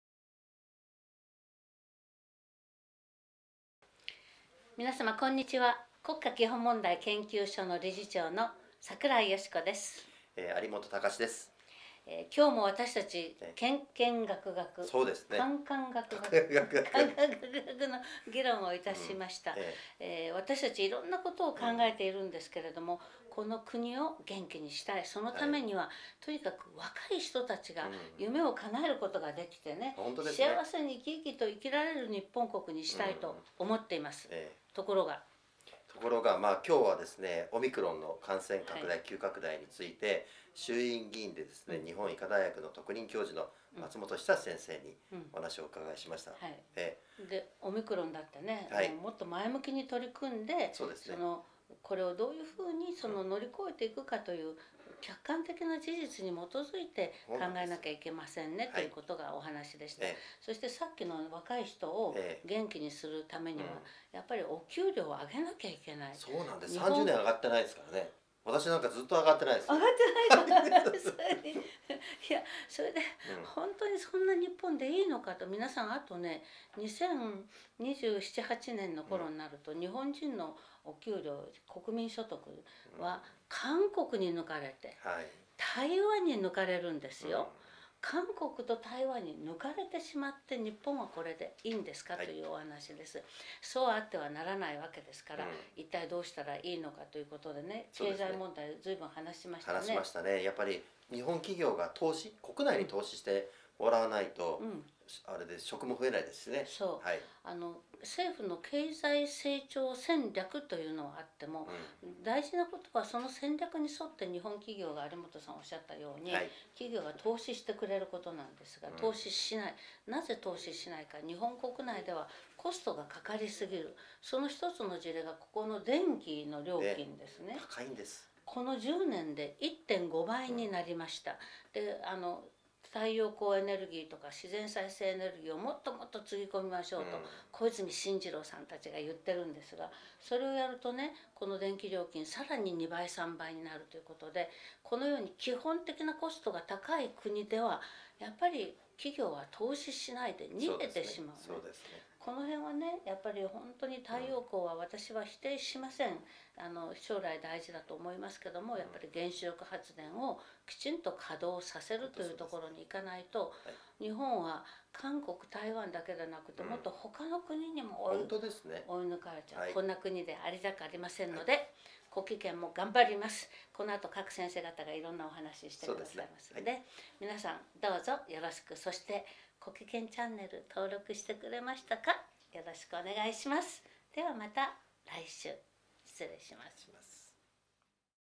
国基研チャンネル 第63回 本日の企画委員会を総括！松本尚議員の説明を受けて
『国基研チャンネル』第63回は、櫻井よしこ理事長と有元隆志さんが、本日の企画委員会を総括。松本尚議員の説明を受け、緊急医療体制の在り方やオミクロン株のことも勉強しました。日本の若者を元気にしたい。そのため経済の活性化は必須です。ともに頑張りましょう。